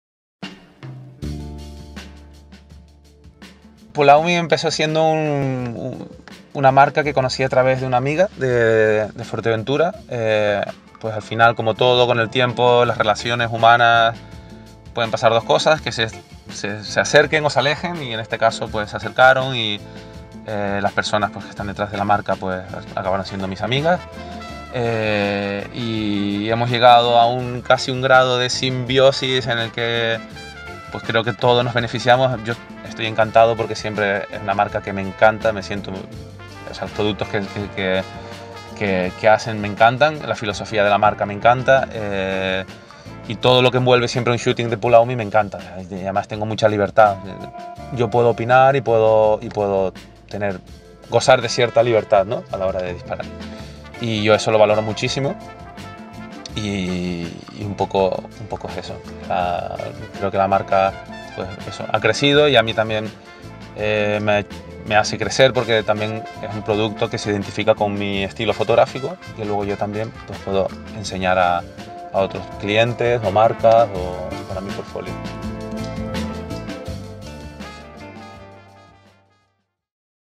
Pulaumi Souls - Behind the scene at the Pulaumi shooting with Sergio Villalba
I had the pleasure to be at the funky town collection shooting from Pulaumi and watch over the shoulders of Sergio Villalba, who is an amazing surf and lifestyle photographer.